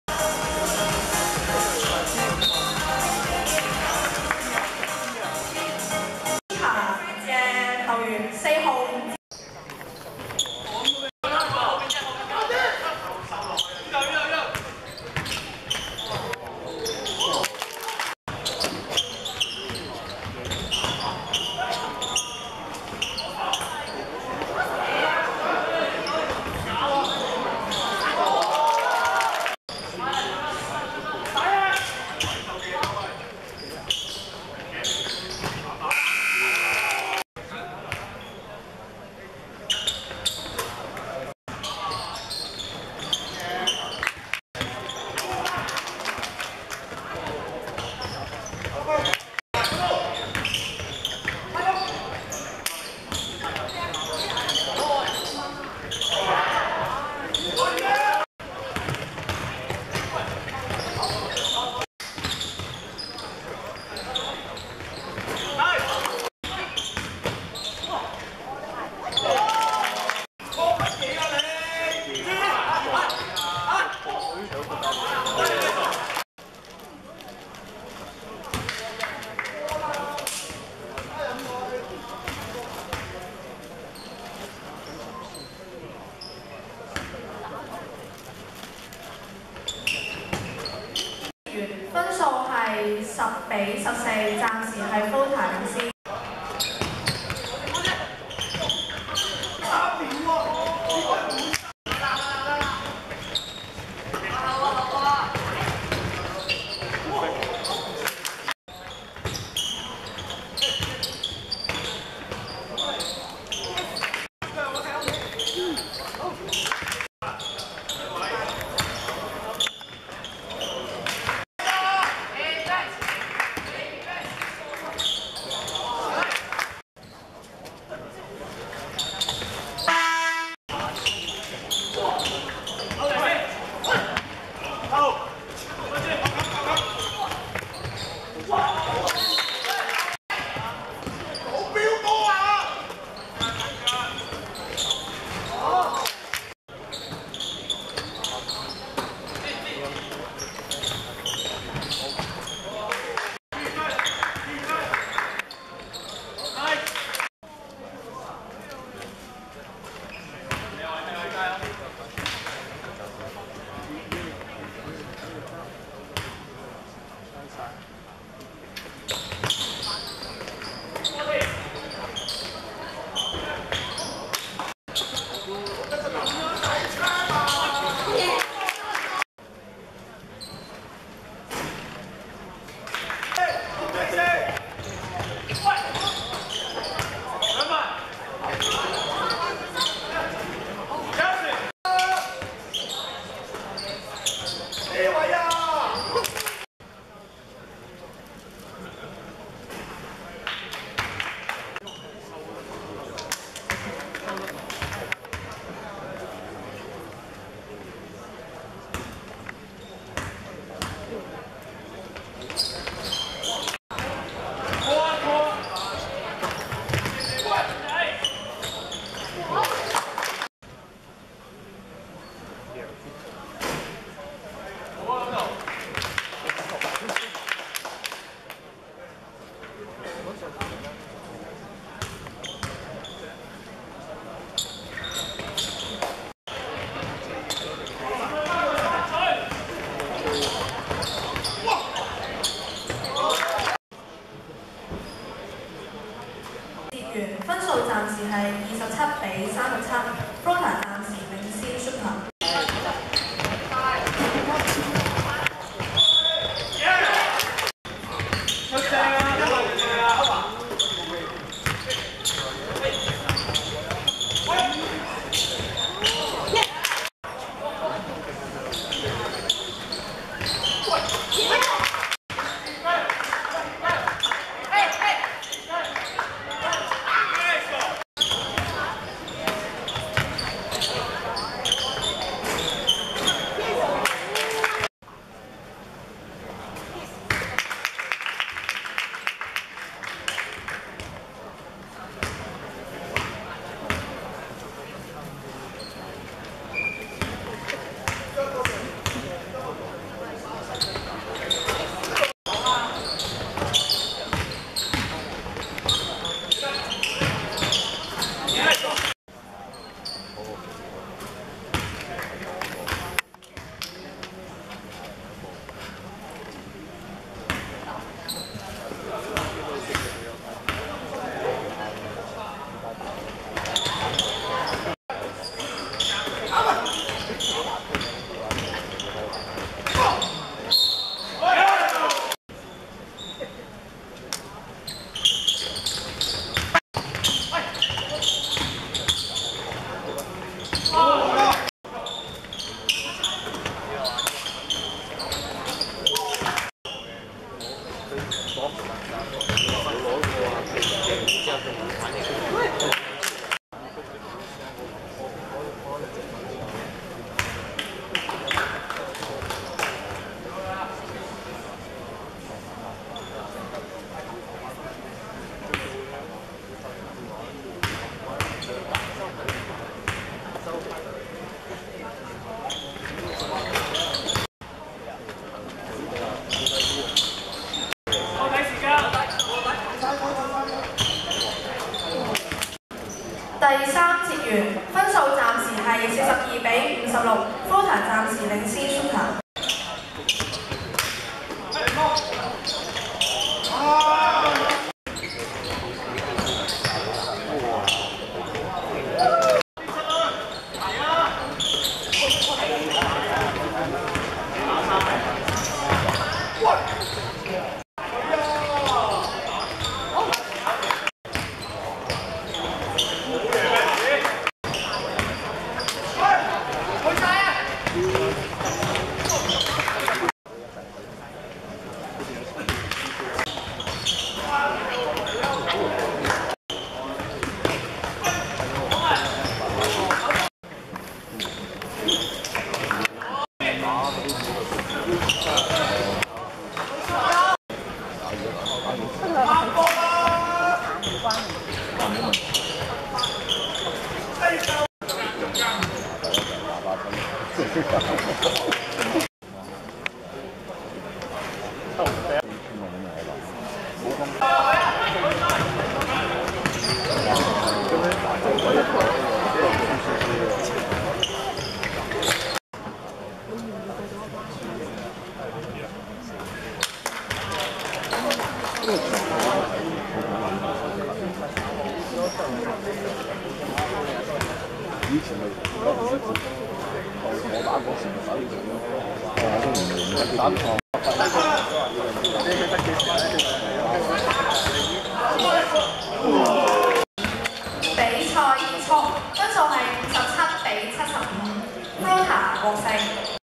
Shooter vs Floater(2014.7.17.Nike League HK U16)Highlights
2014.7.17.Macphers Stadium 15:30 Shooter(white) 57:75 Floater(red)